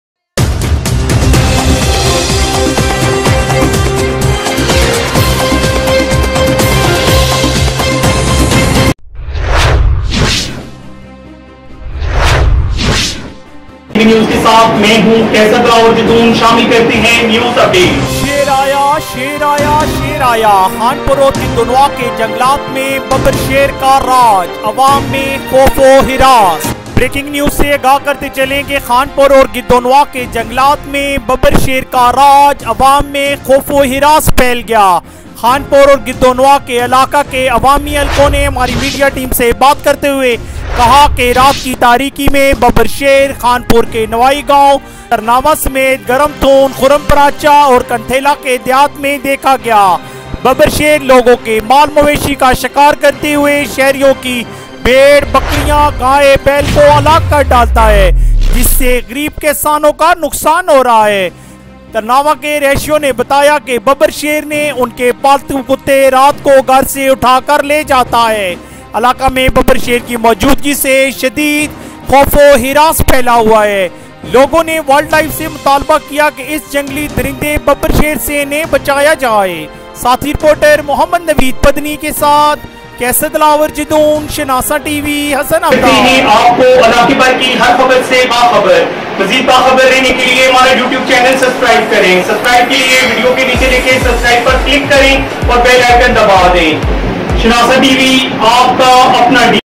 Fear of Lion in rural forests دیہی جنگلات میں ببر شیر کے آنے سے خوف و ہراس.SHANASA TV
Shanasa Tv#Fear#Lion#Rurnal#Forests#Khanpur#Tarnawa#World Life#Local News#Hassan Abdal#Attock#

Fear of Lion in rural forests
دیہی جنگلات میں ببر شیر کے آنے سے خوف و ہراس
Newsrepoter:Qaisar Dilawar Jadoon
Shanasa tv
Hassan Abdal ,Attock
Punjab,Pakistan